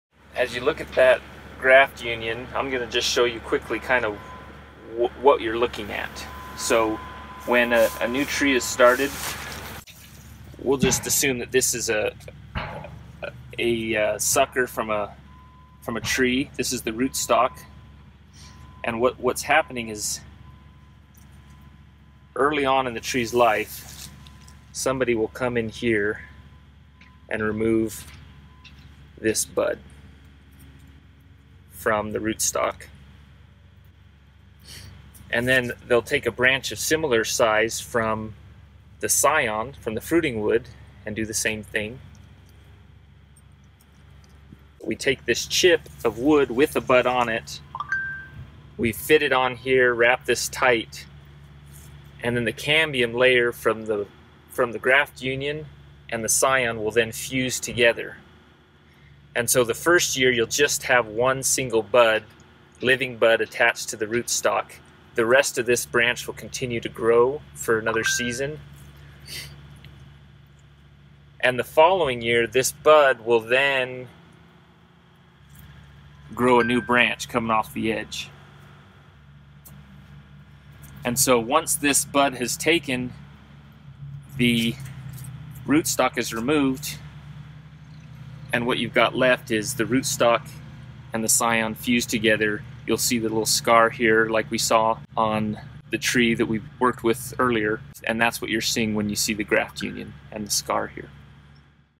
What is a Graft Union?
This video will show you how fruit trees are a combination of a rootstock and a scion and how this process will help you identify a graft union.